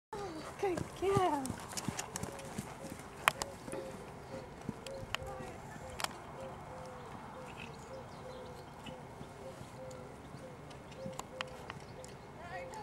PENNY CAN WORK IN AN OUTLINE!!!!!
might not seem like much to you, but it means so much to me, this is 2 years of hard work, finally paying off!